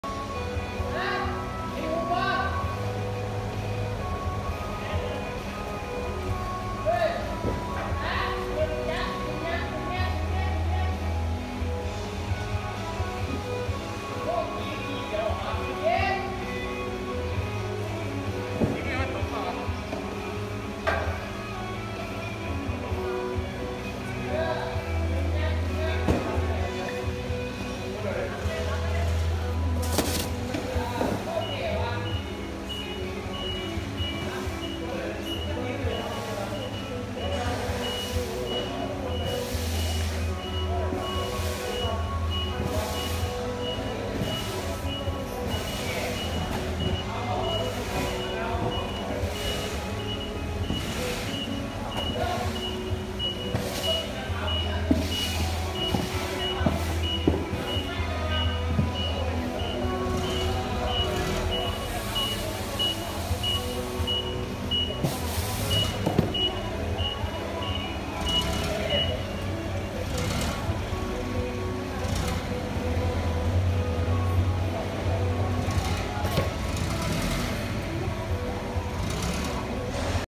Forklift Unicarriers 2.5 ton test load.
CB Truck 1F2 Gasoline / LPG dual ( GK21 ) Model P1F2A25WU # Mast type 2W 500 # Test load weight 1600 kg. Loda center 500 mm. on the floor hight 4500 mm.